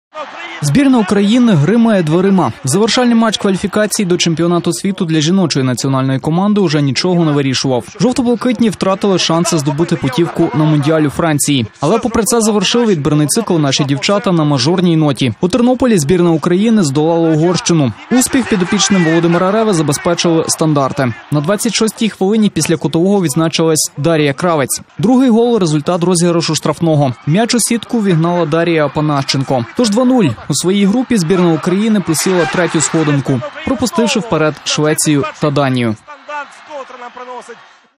FIFAWWC 2019 Qualifier: Ukraine - Hungary - 2:0
⚽ КС2019. 🇺🇦 ️УКРАЇНА - 🇭🇺️УГОРЩИНА - 2:0.
Ukraine 2 - 0 Hungary / Ukrajna–Magyarország 2–0
UEFA Women's World Cup Qualifier 2019

Україна — Угорщина — 2:0 (2:0)
04 вересня. Тернопіль. Стадіон «Міський».
Голи: Кравець 26, Апанащенко 31